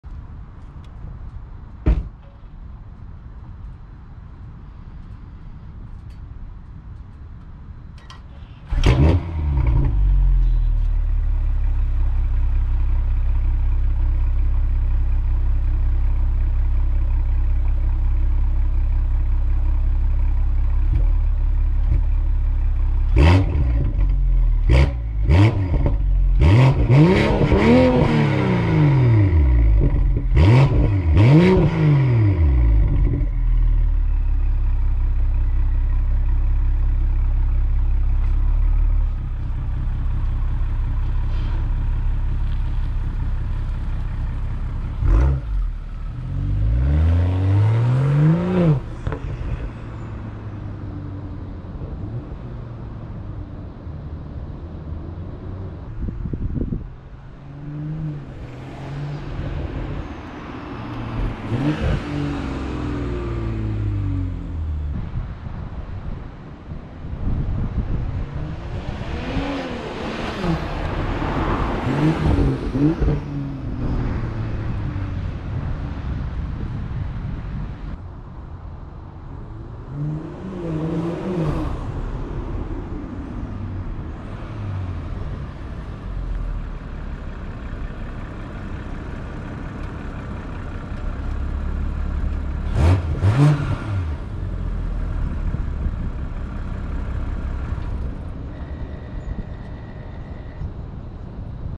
2021 M340i Active Autowerke Catted DP Demo
This is a 2021 M340i with just a Active Autowerke Catted downpipe installed. No other exhaust is on the car and the car is currently not tuned at all so 100% stock other than DP. It really brings the sound out of the B58 engine so I would highly recommend this downpipe especially considering that there is  no CEL as well.

Planning on sending my DME out so I can tune it the right way (No JB4).